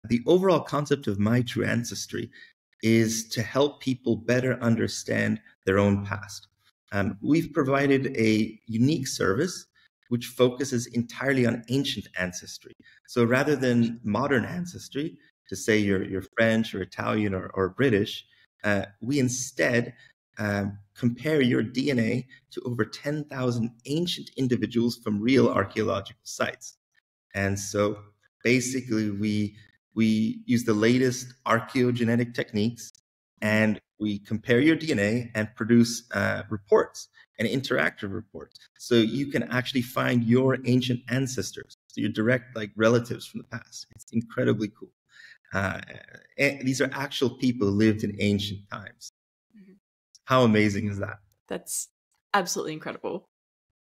Ancient vs Modern Ancestry - Markus Kangas of My True Ancestry
My True Ancestry's mission is to help you take your DNA results a step further. Discover your ancient relatives by comparing yourself to thousands of ancient samples from real archaeological sites. A simple and secure service puts 10,000 years of history, and over 85 ancient civilizations from around the world, at your fingertips.

Have you done a DNA test to find out more about your ancestors and your roots? To discover who you really are, you need to know where you come from. My True Ancestry can take your DNA results one step further through the use of advanced archaeogenetics.

— CHAPTERS IN THE FULL INTERVIEW—
00:00 Intro
0:42 What is MyTrueAncestry.com About?
1:40 How Does It Work?
2:44 How Do You Get Access To Ancient DNA?
4:35 The Interactivity Of The Website
6:24 Can Testing Results Change Over Time?
8:09 Why Did You Start MyTrueAncestry.com?
10:25 New Features: Historic Maps & MySaga.com
16:45 Future Plans With MyTrueAncestry.com
19:12 Outro